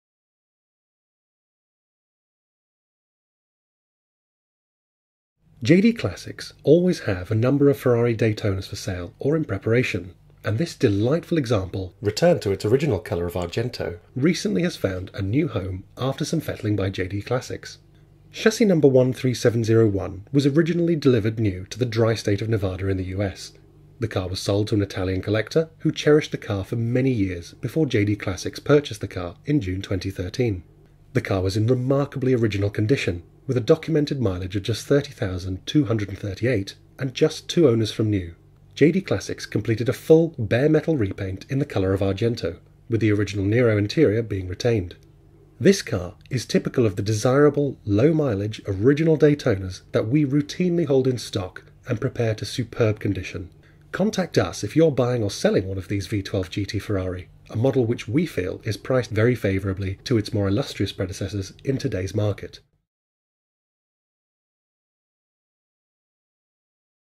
Ferrari Daytona - J.D Classics
JD Classics always have a number of Ferrari Daytonas for sale or in preparation - and this delightful example finished in Argento recently has a found a new home after some fettling by JD Classics.

Chassis number 13701 was originally delivered new to the dry state of Nevada in the US. The car was sold to an Italian collector, who cherished the car for many years before JD Classics purchased the car in June 2013.

The car was in remarkably original condition, with a documented mileage of just 30,238  and just 2 owners from new. JD Classics completed a full bare metal repaint in the colour of Argento - with the original Nero interior being retained.

This car is typical of the desirable low mileage original Daytona's that we routinely hold in stock and prepare to superb condition. Contact us if you are buying or selling one of these V12 GT Ferrari - a model which we feel is priced very favourably to its more illustrious predecessors - in today's market.